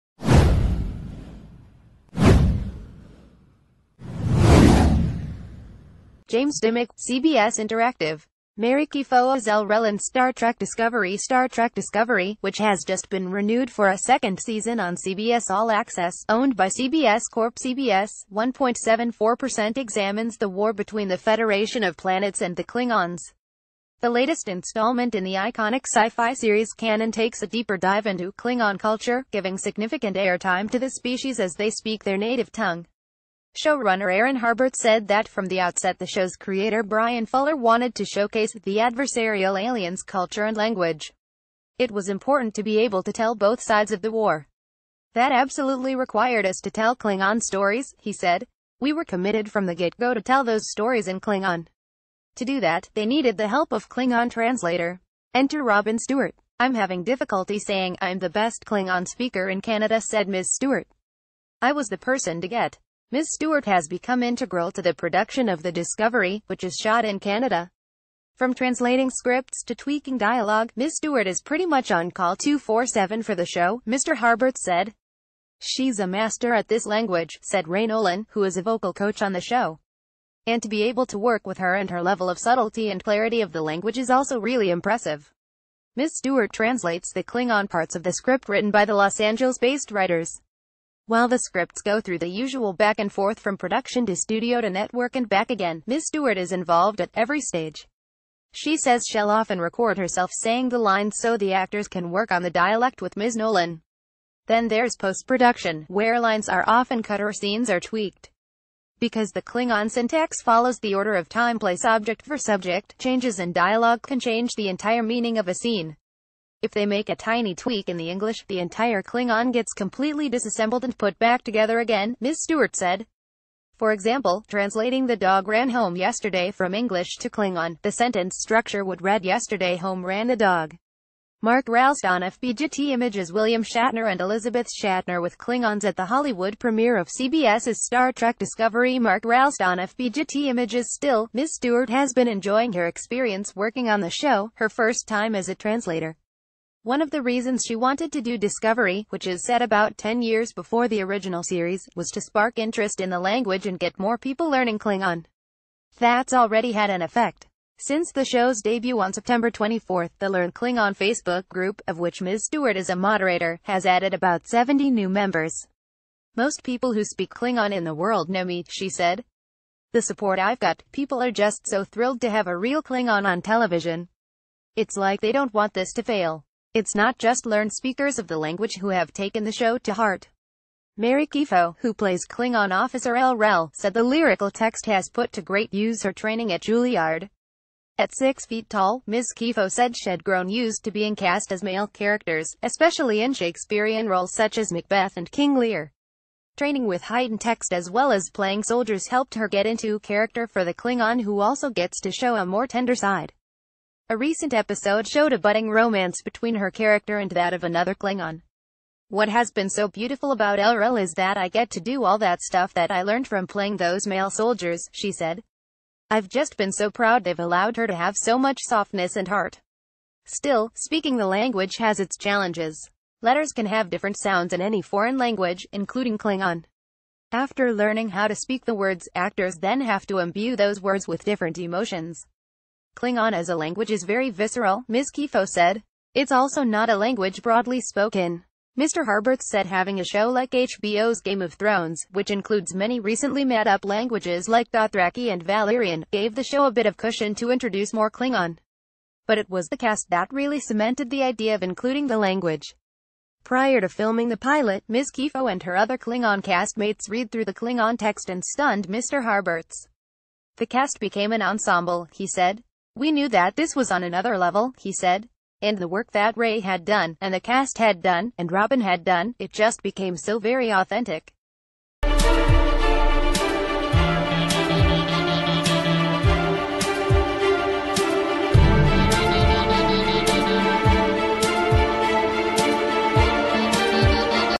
Breaking News | How ‘star trek: discovery’ led to klingon culture finally being embraced
How ‘star trek: discovery’ led to klingon culture finally being embraced

James Dimmock, CBS Interactive. Mary Chieffo as L’Rell in ‘Star Trek Discovery’ “Star Trek Discovery...